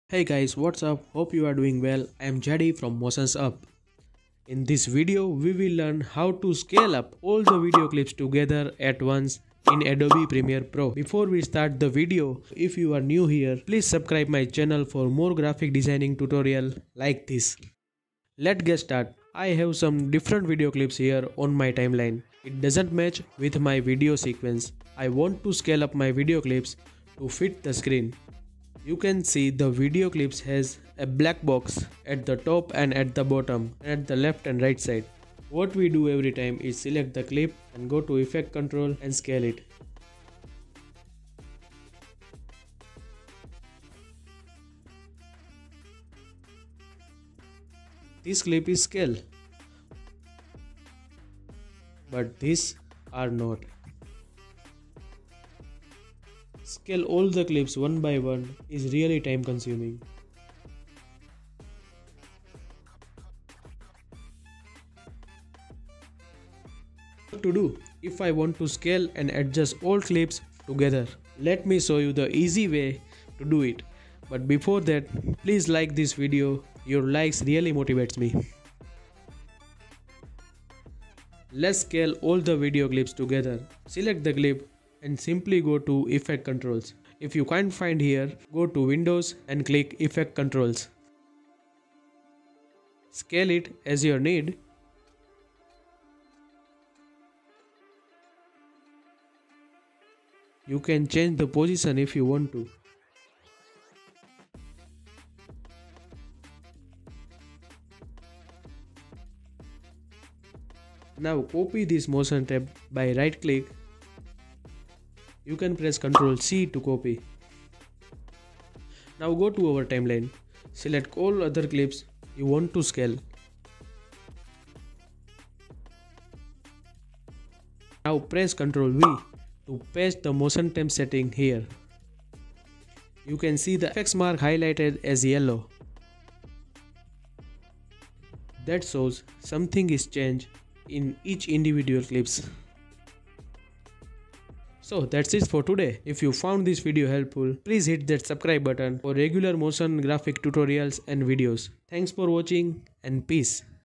How To Scale Multiple Clips At Once | Adobe Premiere Pro Tutorial
How To Scale Multiple Clips At Once In Adobe Premiere Pro Tutorial

► ABOUT TUTORIAL ▶️
In this Adobe Premiere Pro2023 tutorial, Motions Up shows you How To Scale Multiple Clips At Once In Adobe Premiere Pro.  we're going to learn how to Scale Up All Different Video Clips Togather At Once In Simple Steps.